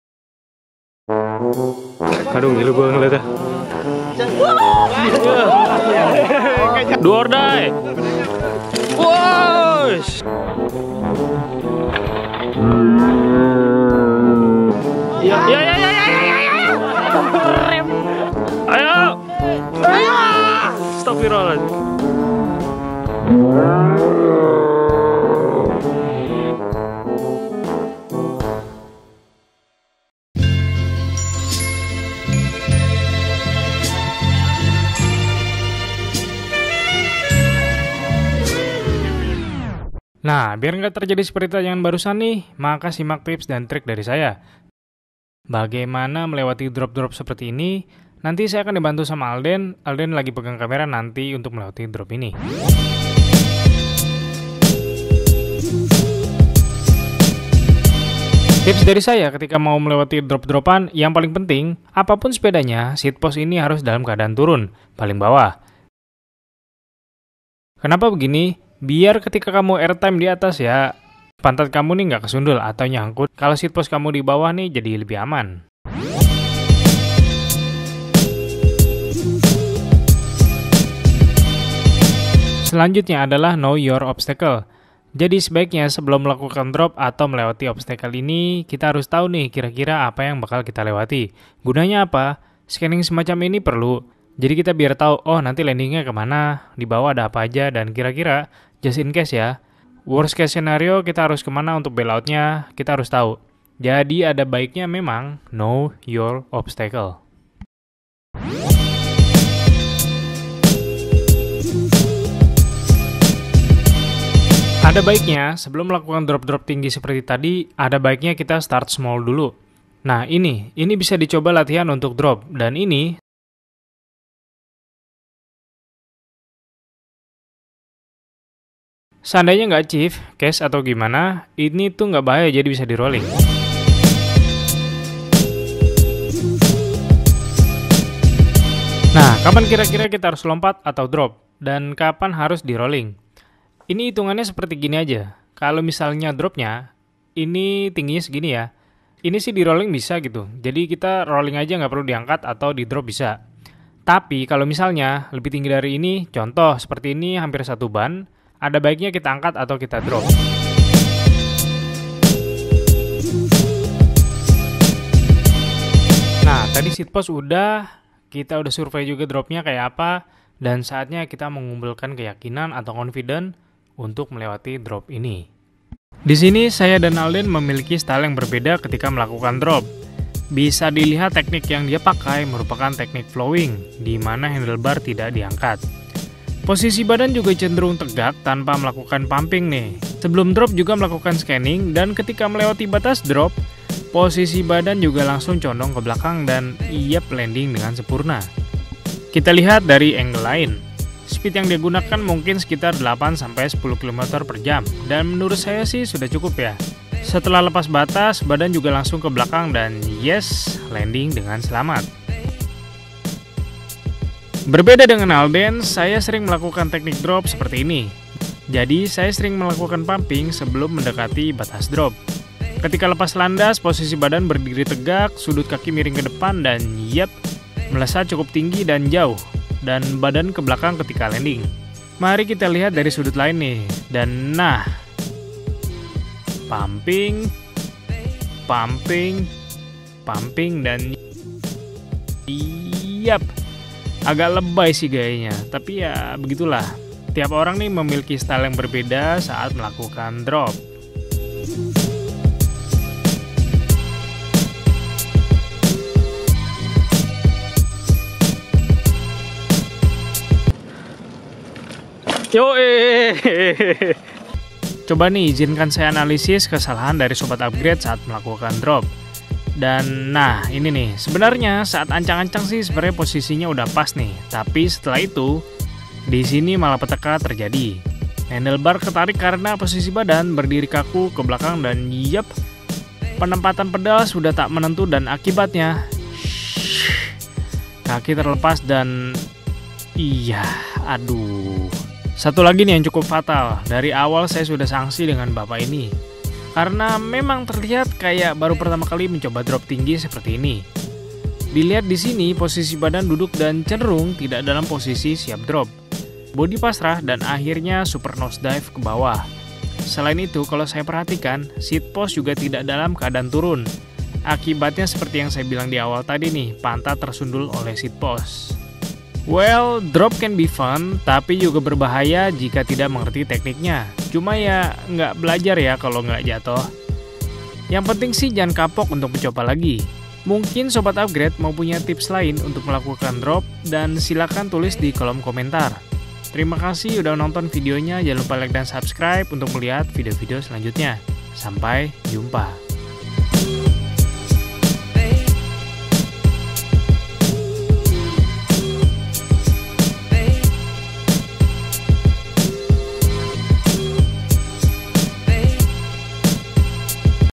Tips & Trick /// Cara Melakukan Drop Di Atas Sepeda
Tidak dijamin! Semua tanggung jawab masing-masing. Well, drop can be fun, tapi juga berbahaya jika tidak mengerti tekniknya, cuma ya, gak belajar klo gak jatoh. Yang penting jangan kapok untuk mencoba lagi. Mungkin sobat upgrade punya tips lain untuk bagaimana cara melakukan drop? silakan tulis dikolom komentar untuk berbagi.

Bike: Polygon Siskiu T8 2018
Camera: Lumix G85
Soft Edit: Premiere Pro CC / After Effect CC